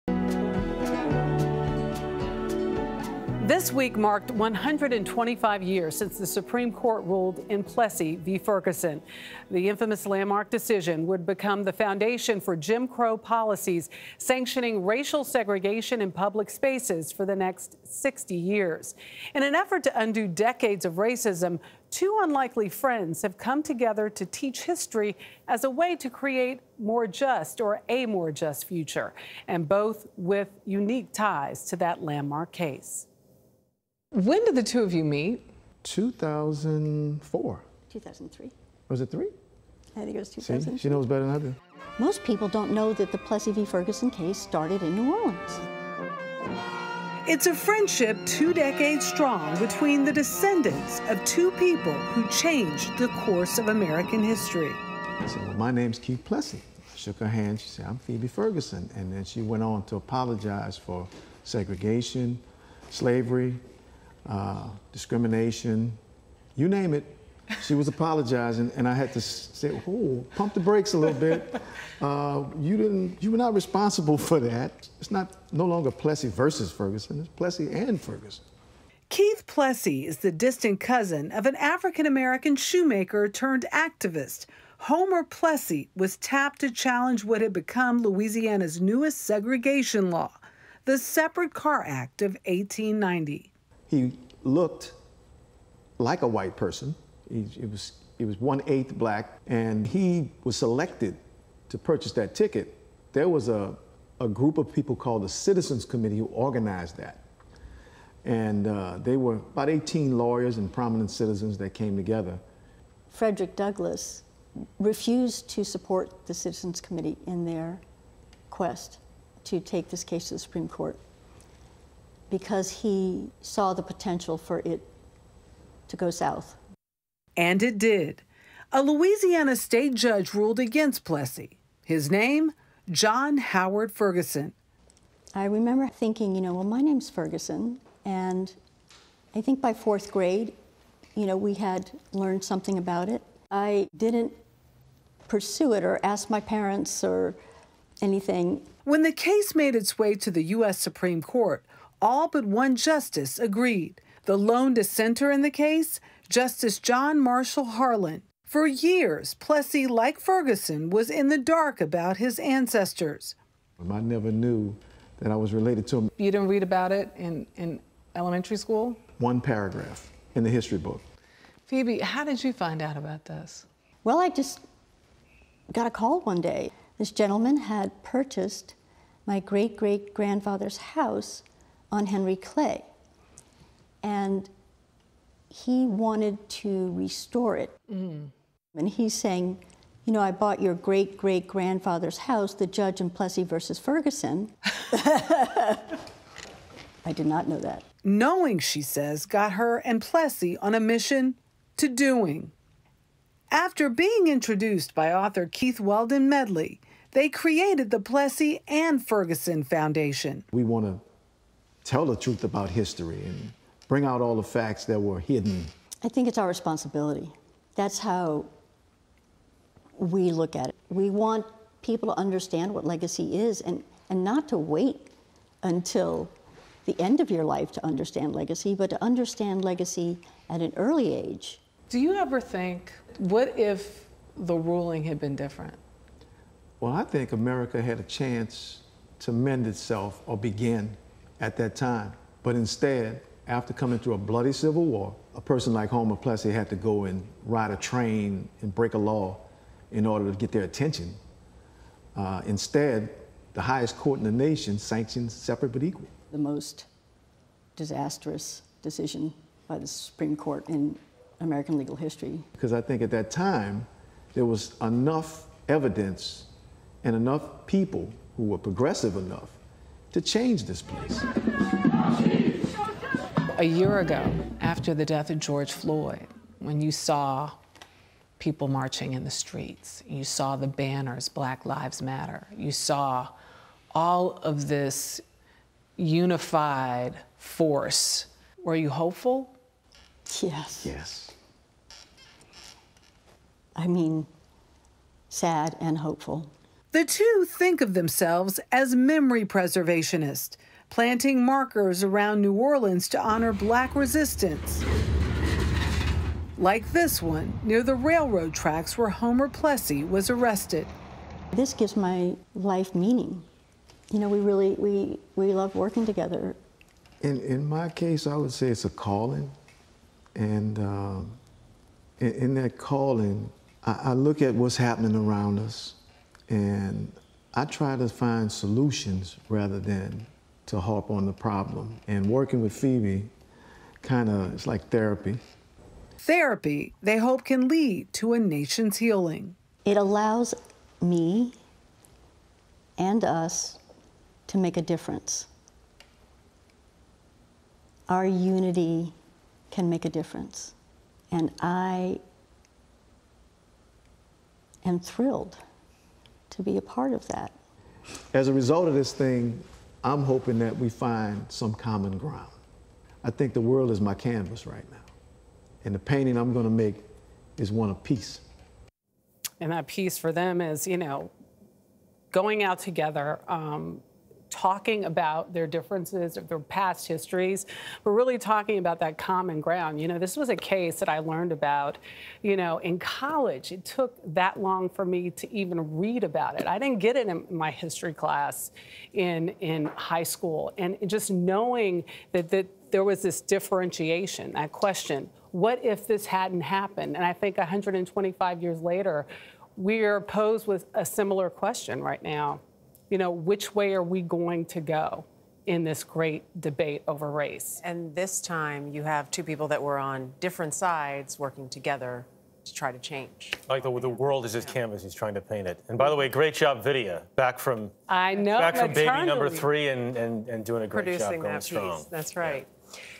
Descendants of Plessy v. Ferguson create unlikely friendship
This week marked 125 years since the Supreme Court ruled in Plessy v. Ferguson. The infamous landmark decision would become the foundation for Jim Crow policies sanctioning racial segregation in public spaces for the next 60 years. In an effort to undo decades of racism, two unlikely friends have come together to teach history as a way to create a more just future. Michelle Miller has the details.